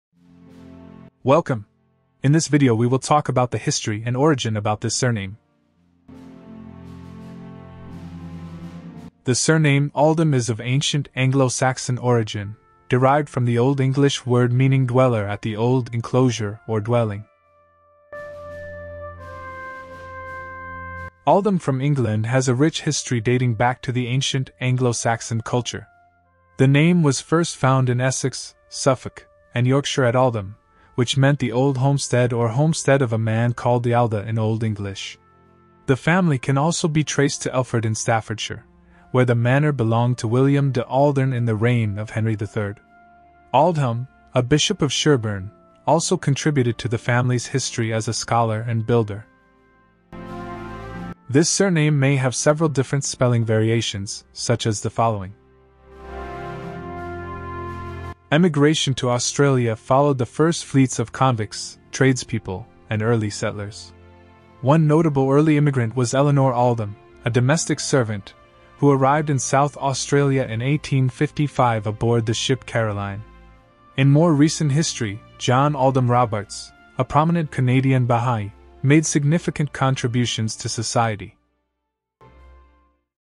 Aldham from England 🏴󠁧󠁢󠁥󠁮󠁧󠁿 - Meaning, Origin, History & Migration Routes of Surname
The surname Aldham is of ancient Anglo-Saxon origin, derived from the Old English word meaning 'dweller at the old enclosure or dwelling.'

Aldham from England has a rich history dating back to the ancient Anglo-Saxon culture. The name was first found in Essex, Suffolk, and Yorkshire at Aldham, which meant 'the old homestead' or 'homestead of a man called Ealda' in Old English. The family can also be traced to Elford in Staffordshire, where the manor belonged to William de Alderne in the reign of Henry III. Aldhelm, a bishop of Sherborne, also contributed to the family's history as a scholar and builder.